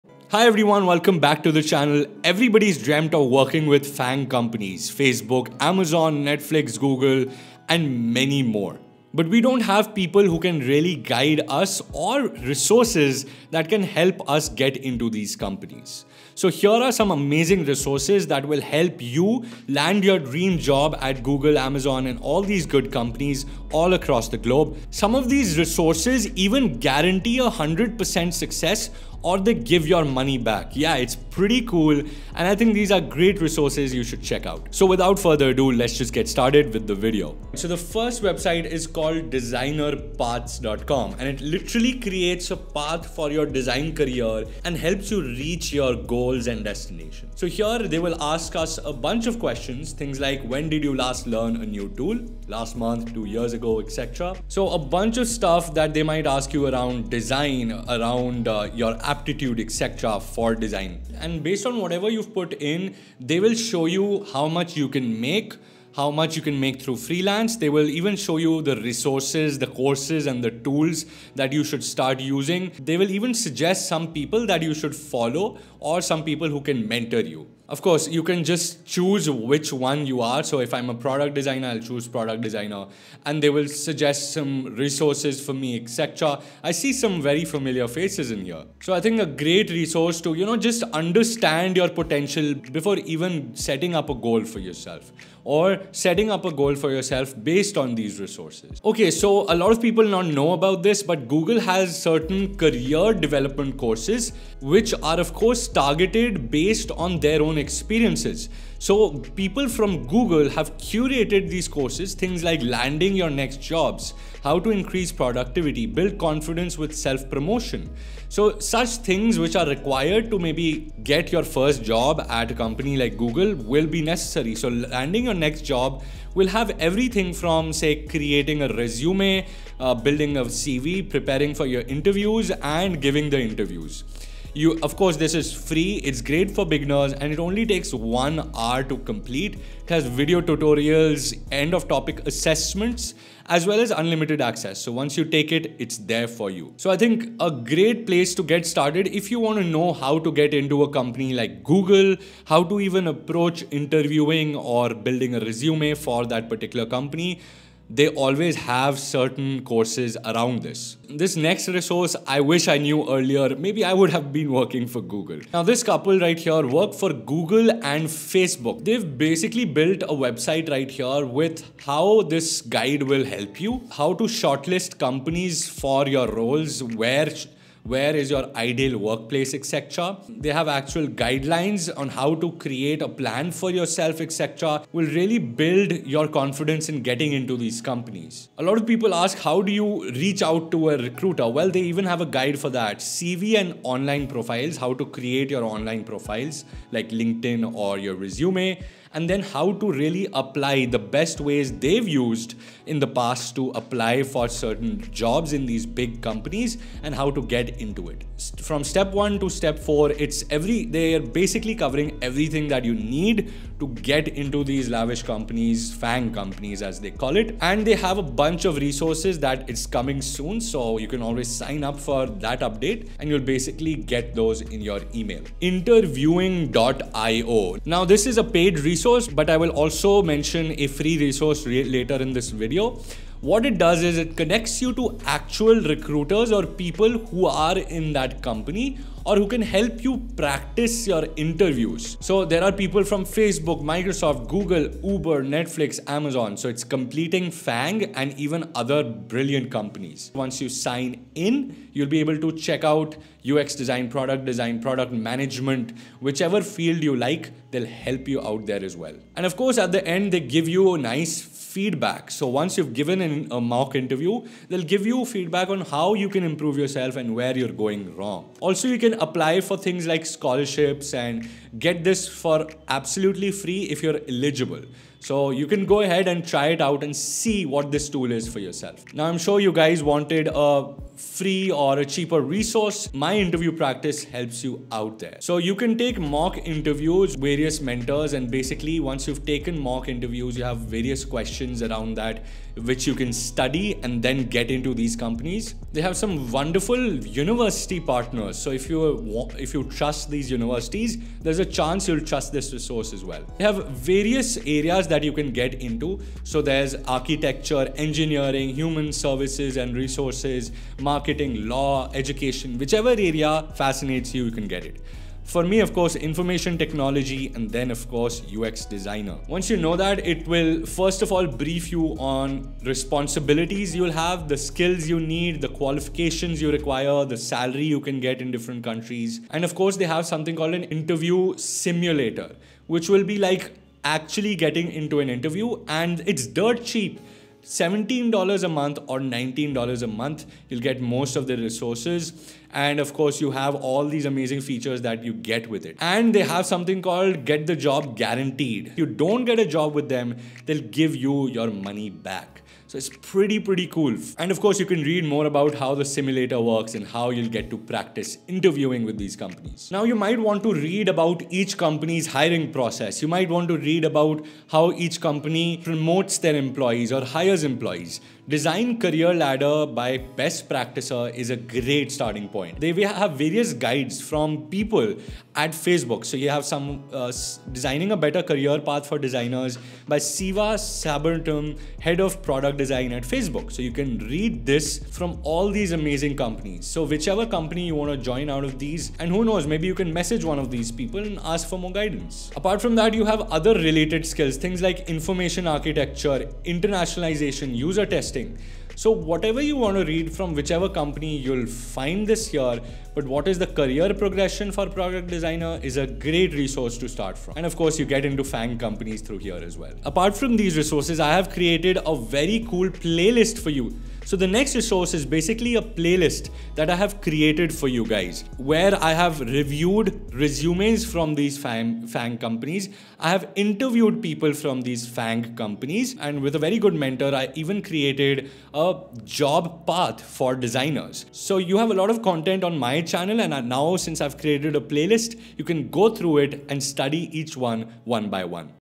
Google's New UX/UI Career Guidance | Interview, Resume, Salary Negotiation | Design Essentials
These UX/UI design resources and tools will help you get your dream  job at FANG companies like Google, Apple, Amazon, Netflix, and more. I wish I had these tools when I was learning design!

Video Chapters
0:00 Introduction
0:37 Designer Paths
1:38 Google's Career Tools
2:41 Work For FANG
3:45 Interviewing IO
4:39 My Interview Practice
5:59 Design Career Ladder
6:49 Special Dream Job Playlist

Get the equipment I use:
1. Mic
2. Camera
3. Macbook Pro
4. Asus Gaming Laptop
5. Pop filter